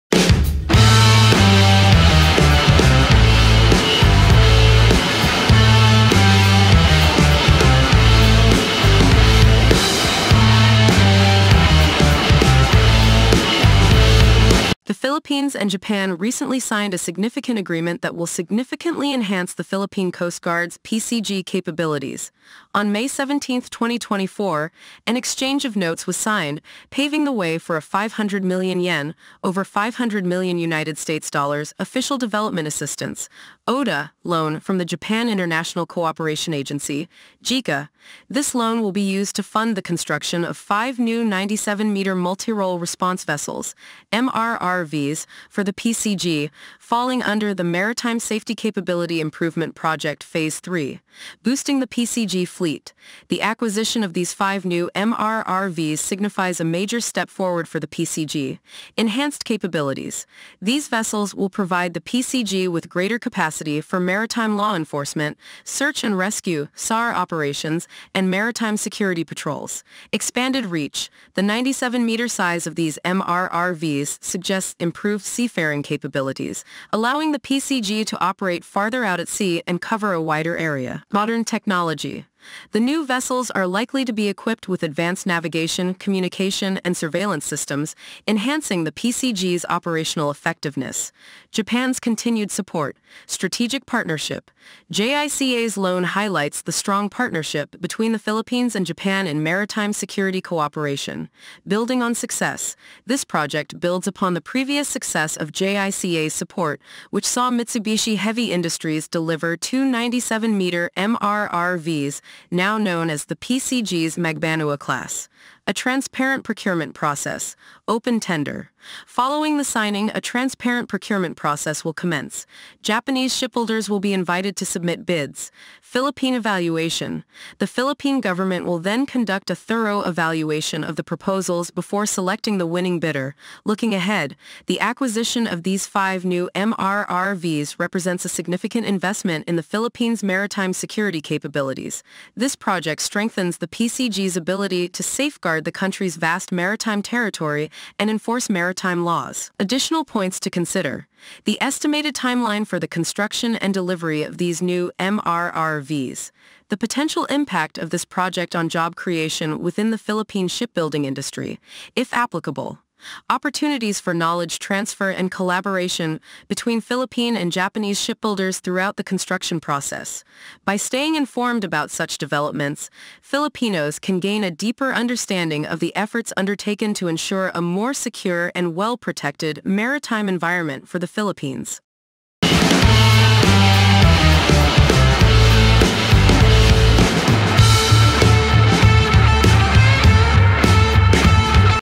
Philippines Coast Guard : Japan Approves Loan for 5 New Multirole Response Vessels
The Philippines and Japan have taken a significant step towards strengthening the Philippine Coast Guard (PCG) with the signing of an Exchange of Notes yesterday, May 17th, 2024. This agreement paves the way for a ¥500 million (over US$500 million) Official Development Assistance (ODA) loan from the Japan International Cooperation Agency (JICA).

Boosting Maritime Safety: The PCG's Gain

This loan will be instrumental in the Maritime Safety Capacity Improvement Phase III Project, allowing the PCG to acquire:

    Five new 97-meter Multirole Response Vessels (MRRVs): These vessels will significantly enhance the PCG's capabilities in various maritime security and safety operations:
        Search and Rescue (SAR)
        Law Enforcement at Sea
        Marine Environmental Protection
        Maritime Security Patrols

Japan's Supportive Role

JICA's loan offers several benefits for the Philippines:

    Favorable Terms: The loan comes with a very low interest rate, making it a financially viable option for the Philippine government.
    Beyond Construction: The loan encompasses not just the construction of the vessels but also integrated logistics support, ensuring long-term operational efficiency.

Collaboration for a Stronger Fleet:

The project signifies continued collaboration between the Philippines and Japan:

    Procurement Process: Japan will offer the project to qualified Japanese shipbuilders for a tender process.
    Evaluation and Selection: The Philippine government will oversee the evaluation and selection of the winning bidder.
    Building on Success: This project follows the successful delivery of two 97-meter MRRVs constructed by Mitsubishi Heavy Industries (MHI), now known as the PCG's Magbanua-class vessels.

Looking Ahead:

The acquisition of these new MRRVs represents a major leap forward for the PCG.  These vessels will:

    Expand the PCG's Operational Reach: The additional vessels will enable the PCG to patrol a wider area of Philippine maritime territory.
    Enhance Response Capabilities: The improved capabilities of the new MRRVs will allow the PCG to respond more effectively to maritime emergencies and security threats.
    Strengthen Philippines-Japan Partnership: This project further cements the strong partnership between the Philippines and Japan in the realm of maritime security cooperation.

The signing of the Exchange of Notes marks a significant milestone in the Philippines' efforts to modernize its Coast Guard fleet. With Japan's support, the PCG will be better equipped to safeguard Philippine maritime interests and ensure the safety and security of its vast maritime domain.